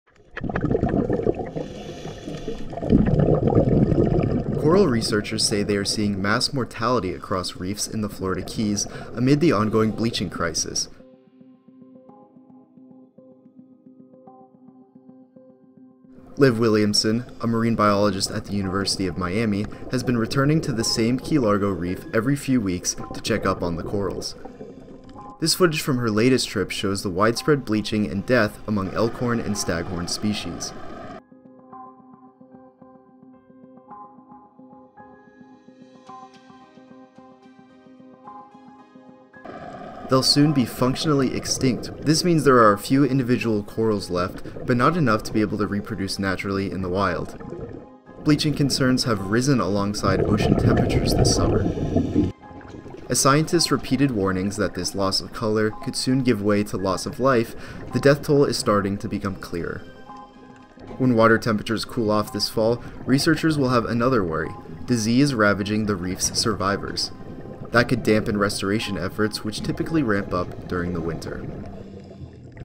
Coral researchers see 'mass mortality' amid Florida Keys bleaching crisis
Battered by heat, washed out to a bleached, white hue and ravaged by disease, corals offshore of Key Largo used what little energy they had left to spawn the next generation that could save their populations.

These elkhorn and staghorn corals — recognized by their iconic branching arms that provide habitat for hundreds of species — are some of the most vulnerable among reefs.

Just weeks after spawning season, more than 90% of those parent corals are dead.

Elkhorn corals are already considered “functionally extinct” in the upper Keys, and other elkhorn and staghorn populations in the Florida Reef are following suit, according to Liv Williamson, an assistant scientist of marine biology and ecology at the University of Miami. That means there are a small number of individual corals left in the only living barrier reef in the continental United States — the world’s third-largest. It stretches about 360 miles from Dry Tortugas National Park to the St. Lucie Inlet — but they can’t reproduce enough in the wild to support a viable population.